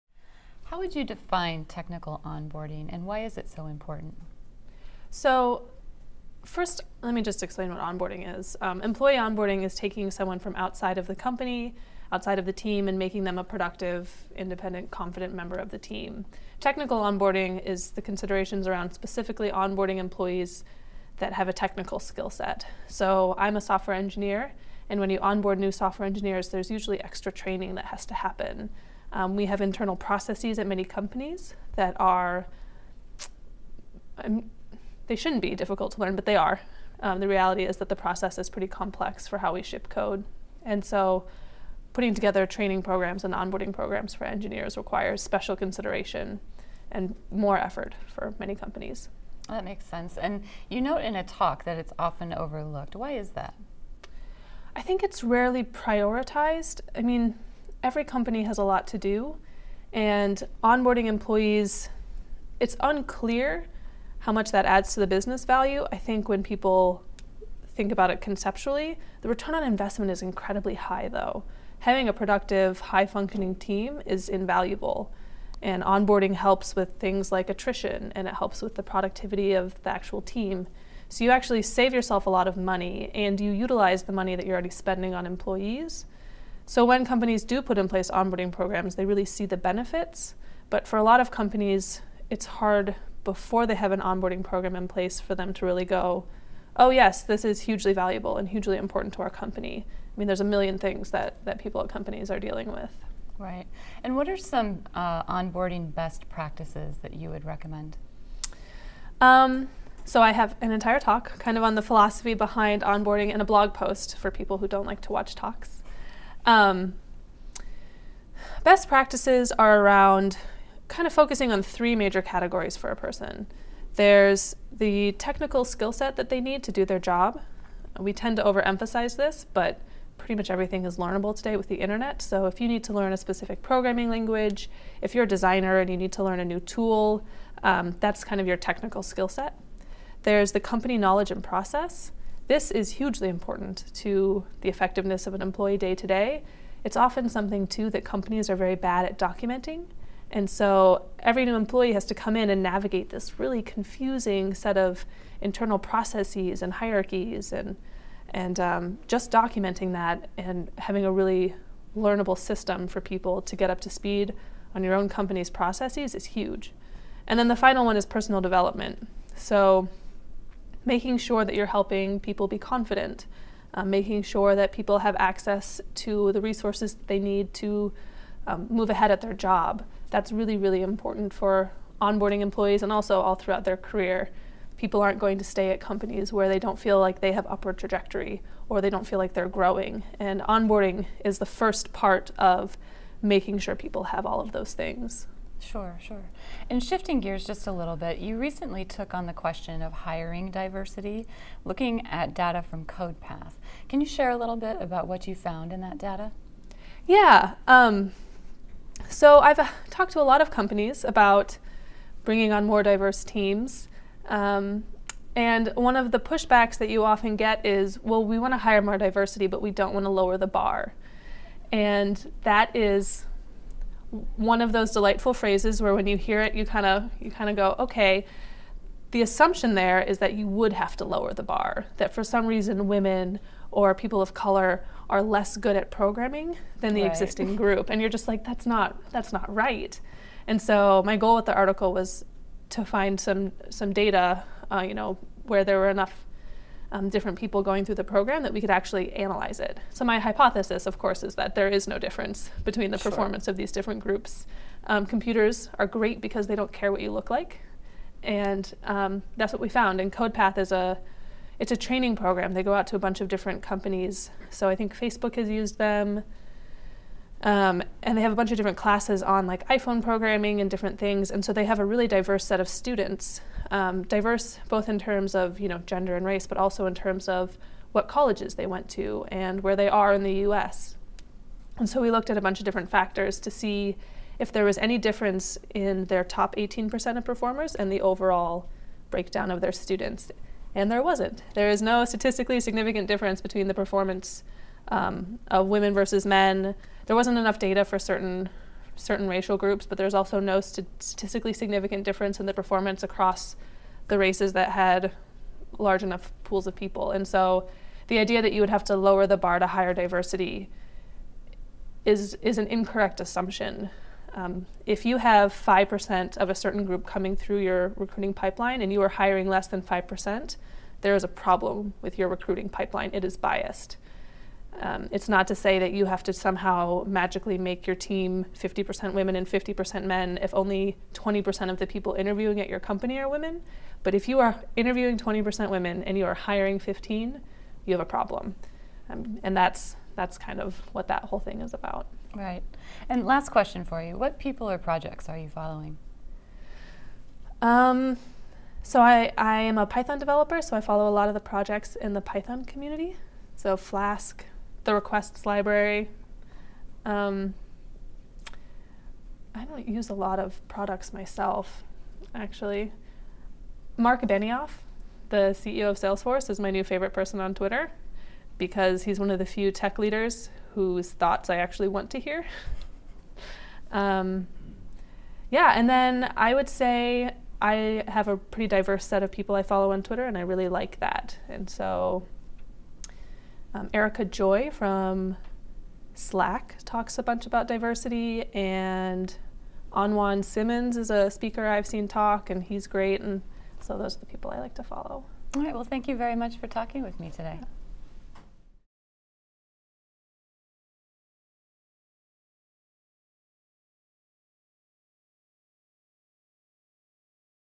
Technical onboarding and hiring diversity - Cultivate interview with Kate Heddleston
Software engineer, blogger, and speaker Kate Heddleston discusses the importance of technical onboarding, and the additional training needed for engineers due to the complicated nature of their work. Kate outlines some of the best practices in the onboarding process, as well as research from CodePath about hiring diversity and overcoming recruiting bias.

Kate Heddleston interviewed by Jenn Webb at Cultivate Strata in San Jose 2016.